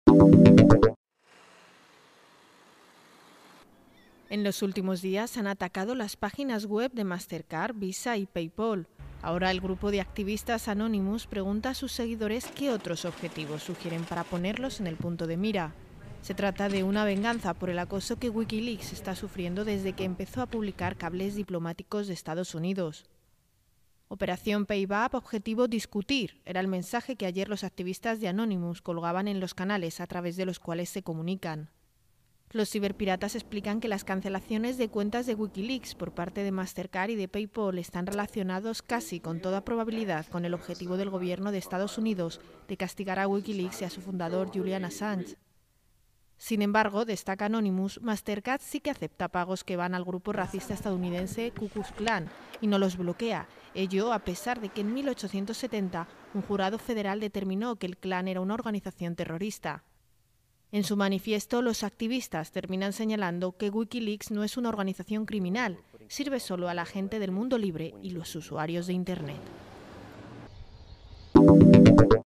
"Anonymus" moviliza la defensa de Wikileaks en Internet
Washington, 12 dic (EFE).- El grupo de activistas "Anonymous" que en los últimos día ha atacado las páginas de internet de Mastercard, Visa y PayPal ha solicitando a sus seguidores sugerencias sobre los próximos objetivos.

"Anonymous" también ha colocado en la red un "manifiesto" en el que justifican su ataque contra Mastercard en "venganza" del acoso que la web WikiLeaks está sufriendo desde que empezó a publicar cables diplomáticos de Estados Unidos.